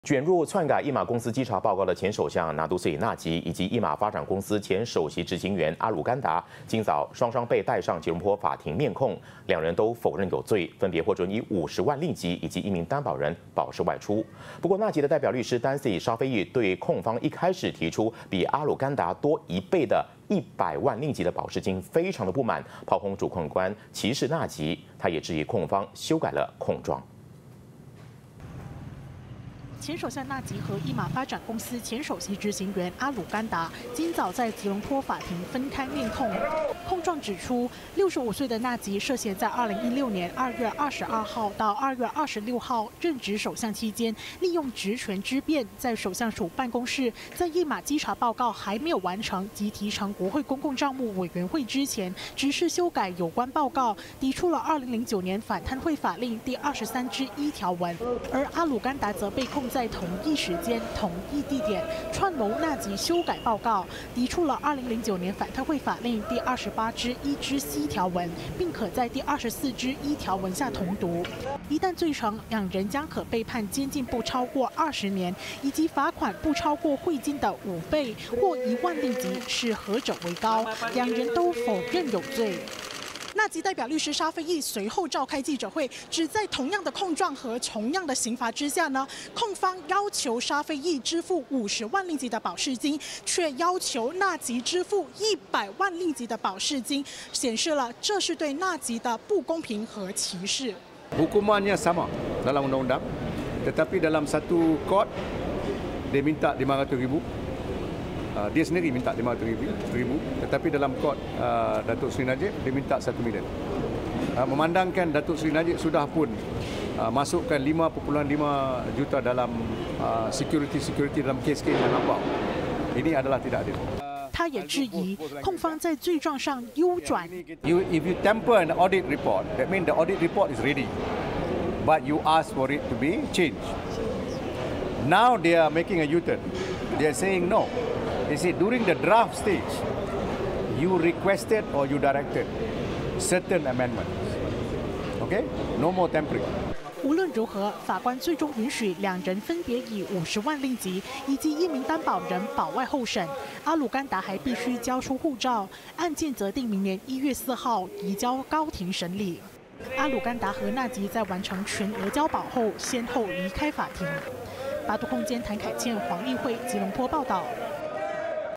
一马稽查报告篡改案 纳吉阿鲁甘达不认罪
卷入篡改一马公司稽查报告的前首相拿督斯里纳吉，以及一马发展公司前首席执行员阿鲁甘达今早双双被带上吉隆坡法庭面控。两人都否认有罪，分别获准以50万令吉，以及1名担保人保释外出。不过纳吉代表律师丹斯里沙菲益对控方一开始提出比阿鲁甘达多1倍的1百万令吉保释金非常不满，炮轰主控官歧视纳吉，他也质疑控方修改了控状。